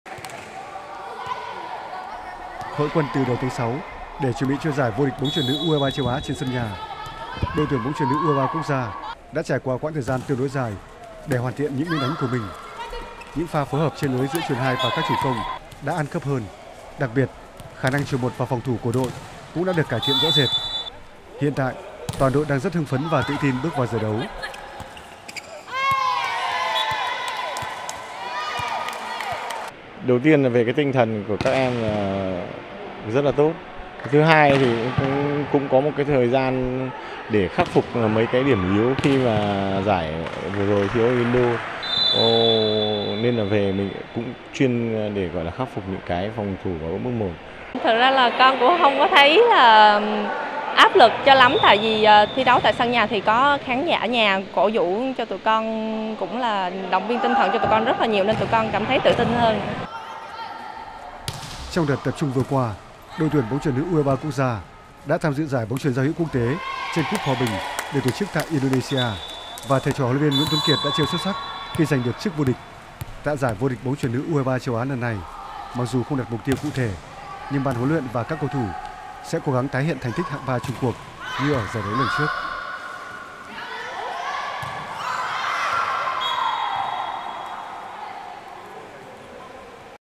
Bóng chuyền Việt Nam | Đội tuyển bóng chuyền nữ U23 Việt Nam tự tin trước giải vô địch U23 châu Á.
Chuẩn bị cho giải đấu một cách chu đáo, đội tuyển bóng chuyền nữ Việt Nam rất tự tin khi bước vào giải trên sân nhà.